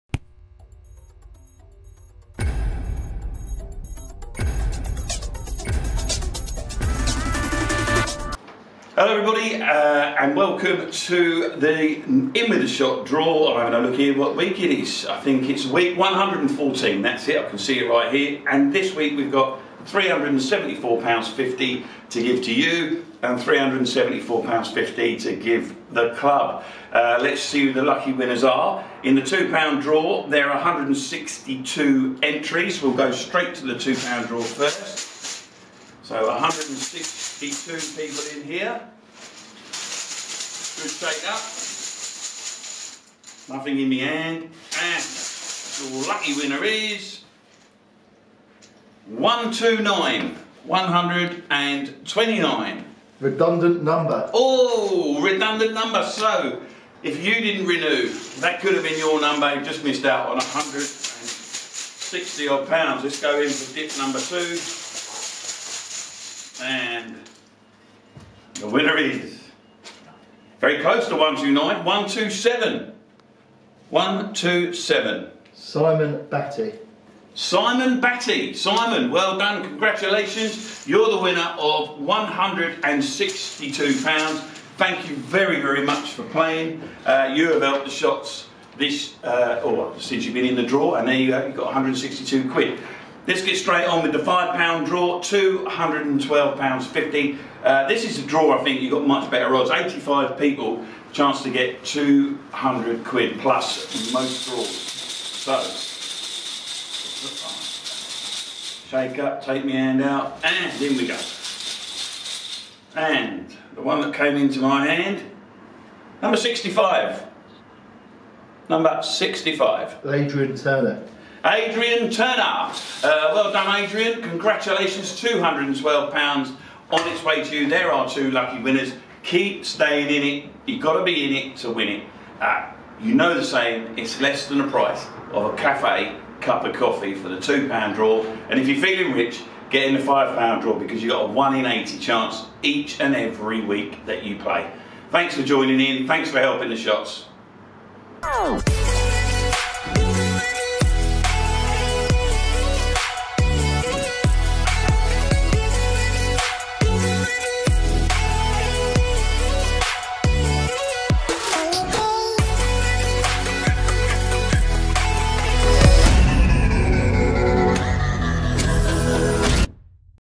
IN WITH A SHOT: Week 114 Winners! (1st April 2022)
The winners of Week 113 Winners (1st April 2022) have been announced by Commercial Manager Mark Butler!